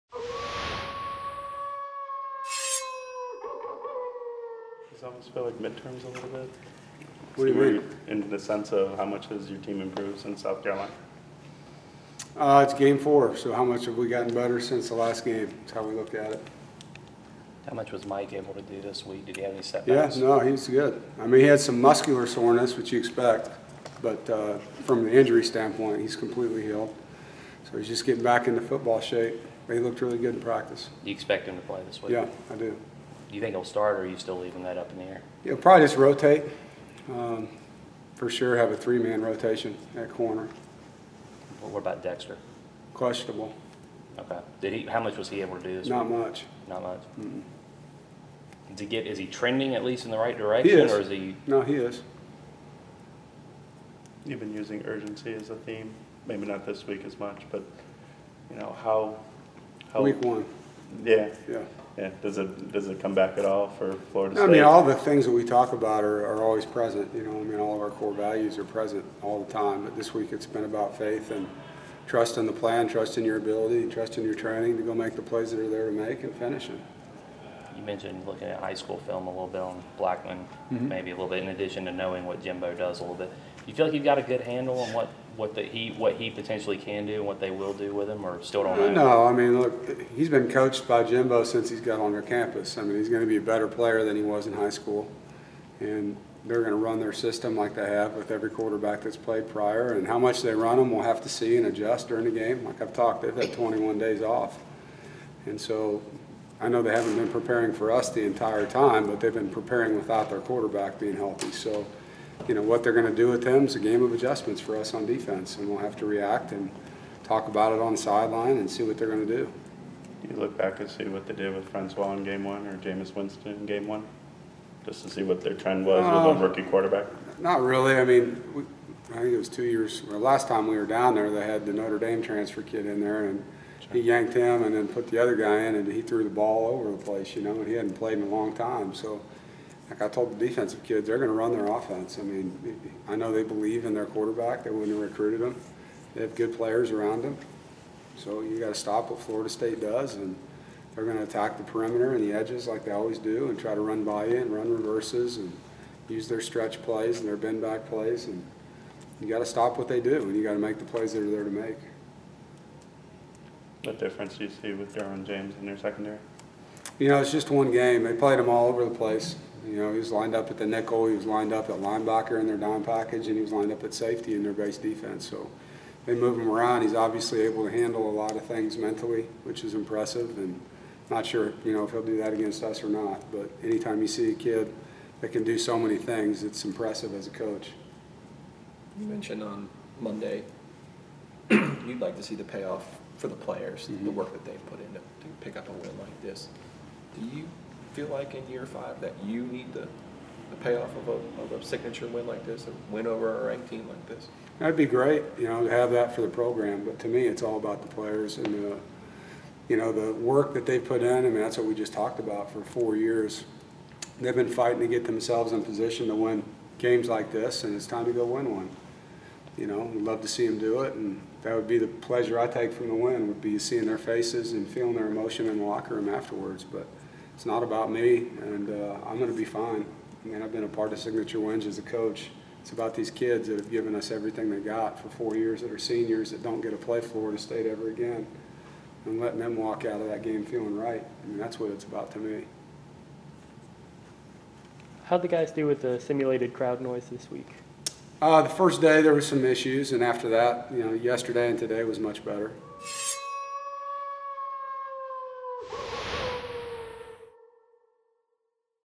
Dave Doeren Media Availability - Sept. 21
NC State head coach Dave Doeren meets with the media before the Pack's trip to #12 Florida State.